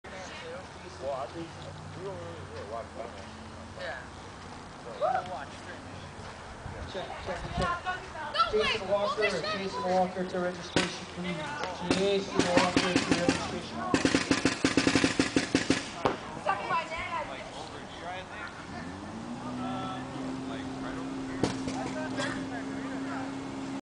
Sawyer having some fun at registration and field walking at the RPL
Kids being kids the day before the games start for the RPL Finale - I love watching Sawyer have fun like this on the fields